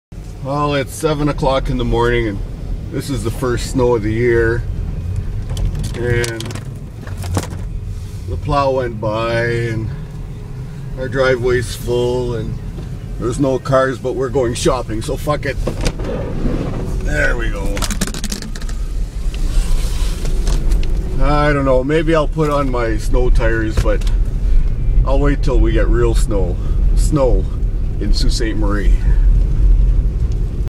First snow of the year
The first real snow of the year.
If this keeps up, I may contemplate putting on snow tires.  Nov 27th 2010